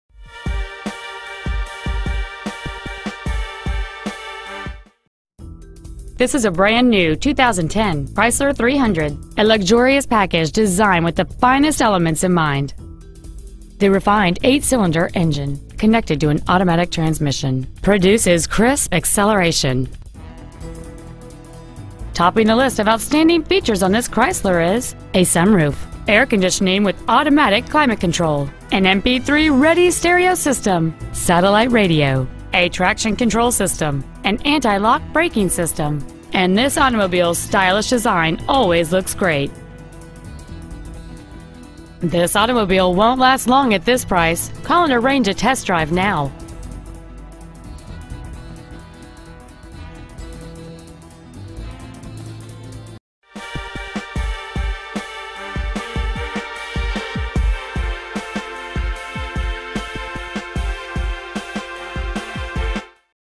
2010 Chrysler 300C Auburn WA
We are proud to present this 2010 Chrysler 300C.

 Year: 2010
 Make: Chrysler
 Model: 300C
 Engine: 5.7L V8
 Trans.: Automatic
 Exterior: Brilliant Black
 Miles: 50
 Interior: Black

 2925 Auburn Way N.

 2010 Chrysler 300C Auburn WA